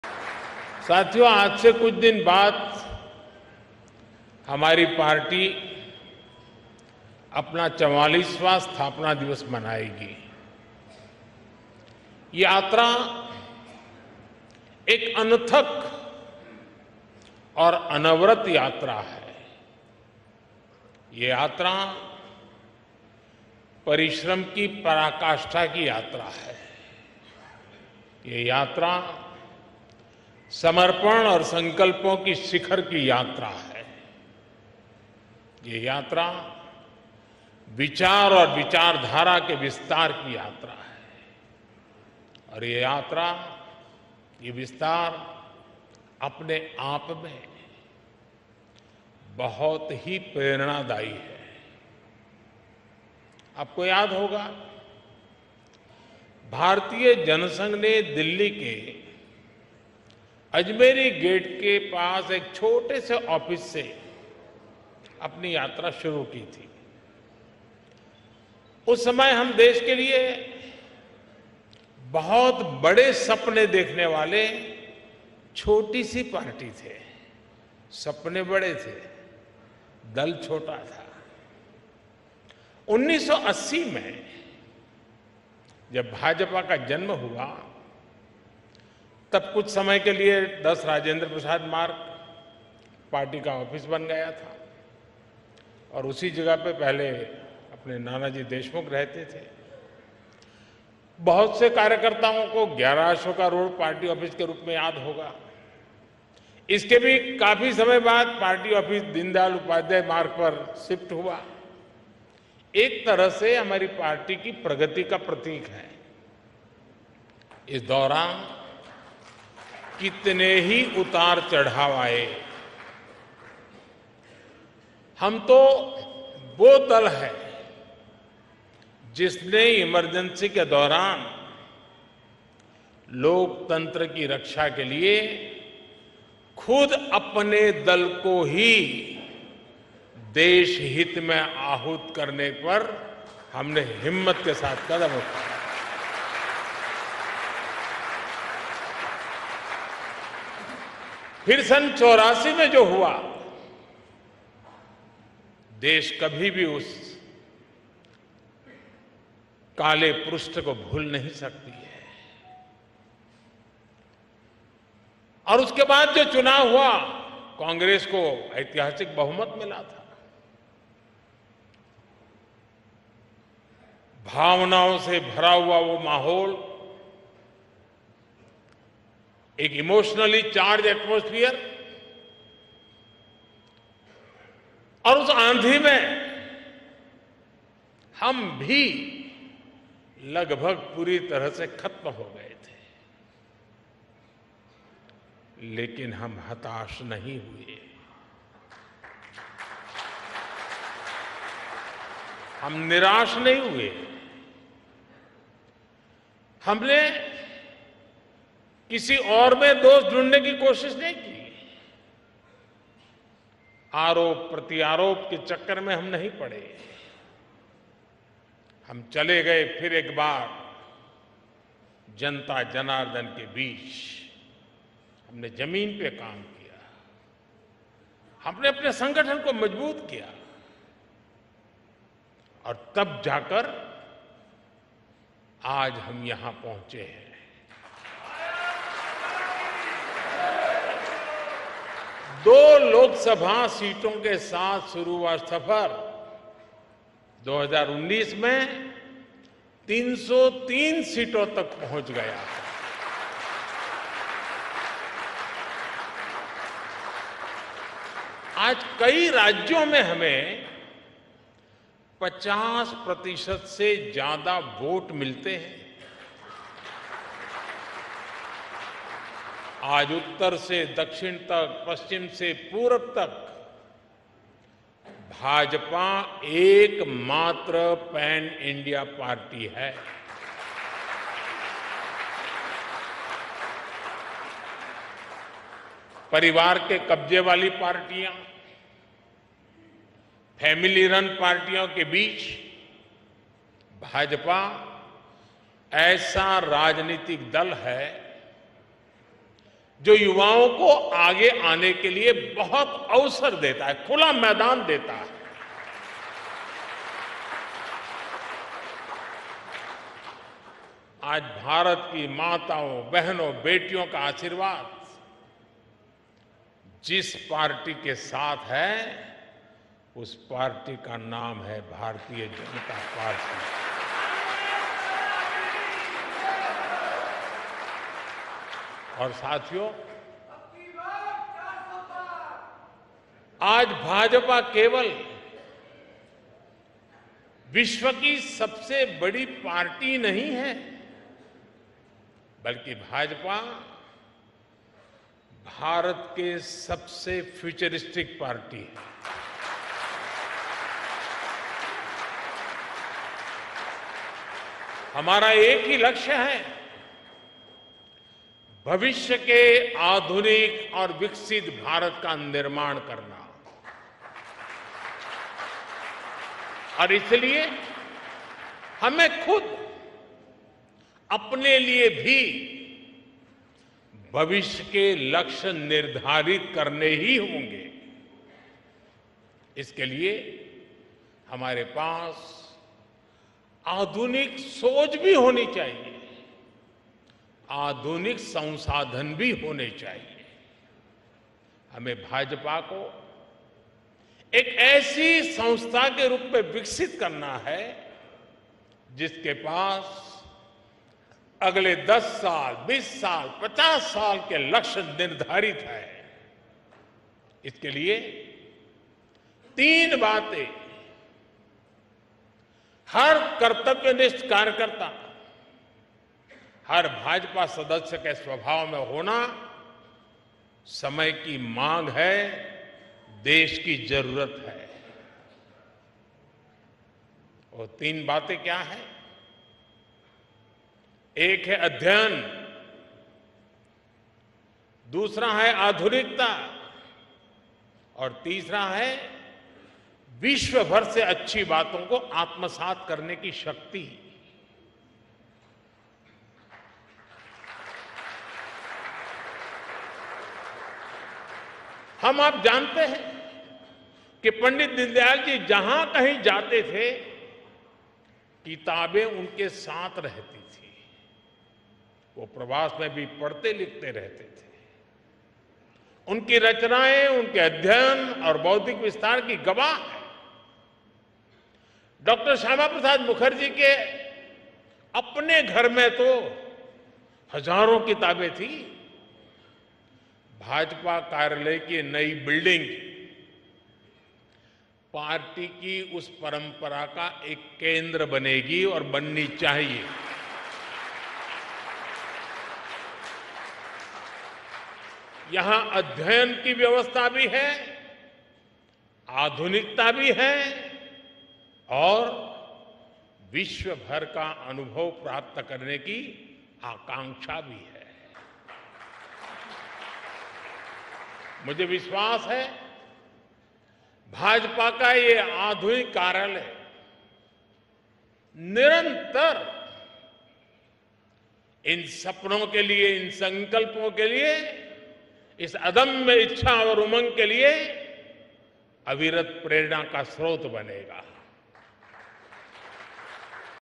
PM Modi ने कहा- हर BJP कार्यकर्ता और सदस्य के स्वभाव में कौनसी 3 बातें जरूर होनी चाहिए!
Prime Minister Narendra Modi addressed party karyakartas at the BJP headquarters during the inauguration of residential complex and auditorium of BJP. Addressing the gathering, he said, “I remember, in February 2018, when I had come to inaugurate this headquarters, I had said that the soul of this office is our karyakartas. Today when we are expanding this office, it is also not just an expansion of a building. Rather, it is an extension of the dreams of every BJP worker, it is an extension of BJP's resolve to serve.”